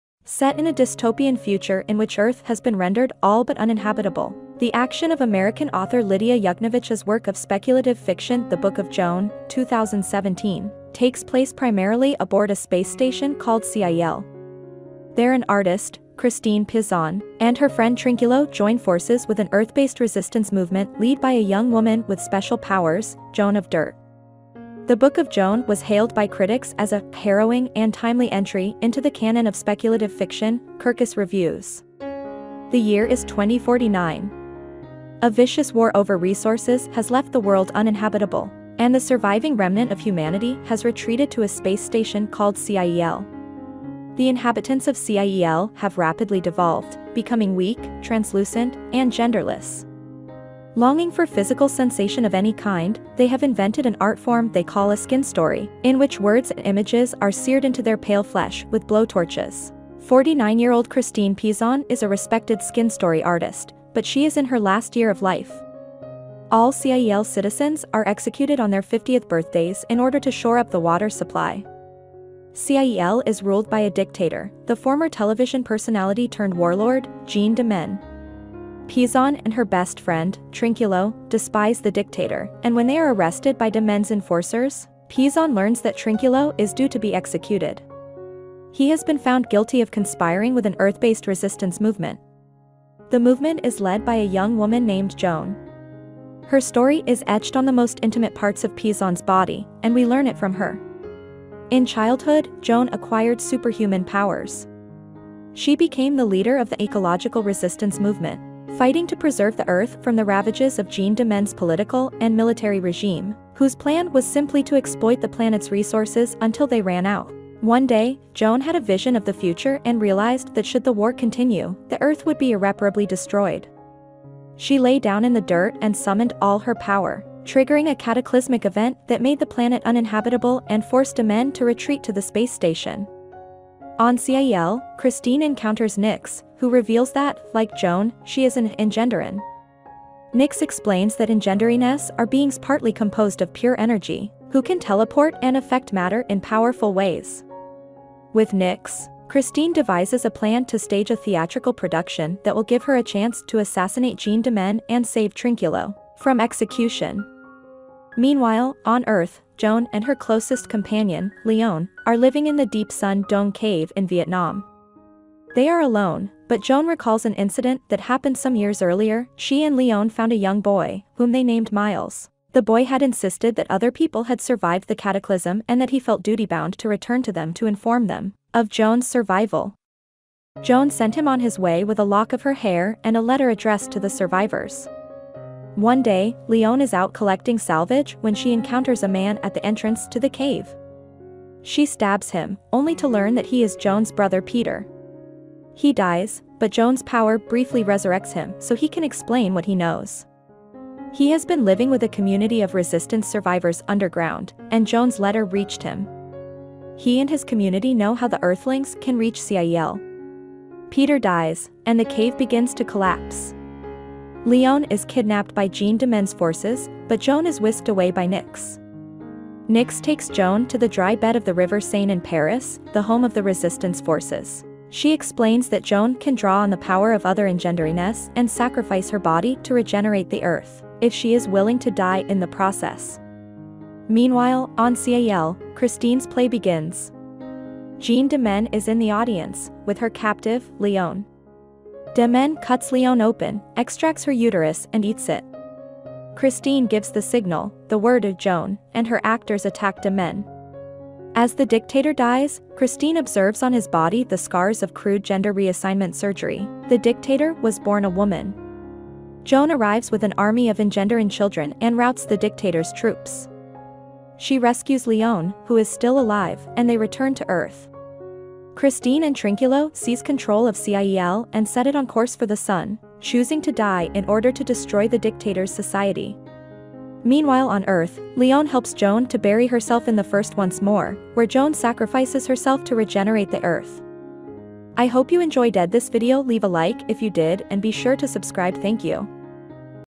Plot summary, “The Book of Joan” by Lidia Yuknavitch in 5 Minutes - Book Review
Set in a dystopian future in which Earth has been rendered all but uninhabitable, the action of American author Lidia Yuknavitch’s work of speculative fiction The Book of Joan (2017) takes place primarily aboard a space station called CIEL.